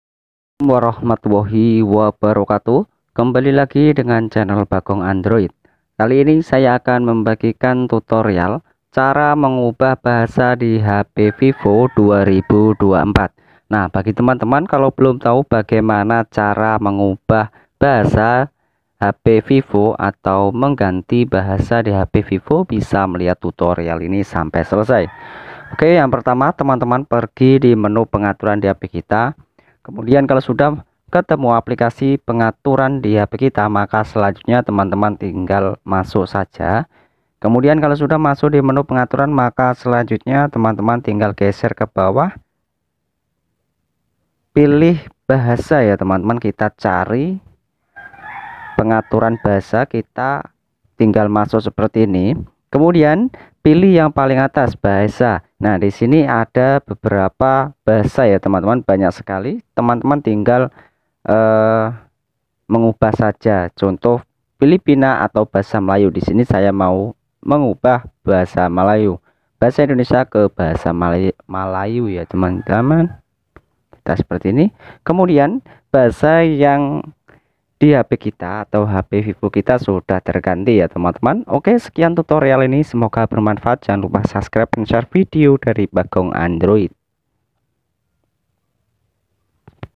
Cara Mengubah Bahasa Hp vivo 2024
Halo teman teman kembali lagi dengan channel Bagong Android kali ini saya akan membagikan Cara Mengubah Bahasa Hp Vivo 2024

Nah Cara Mengubah Bahasa Hp Vivo Terbaru tonton terus video ini sampai selesai agar kalian paham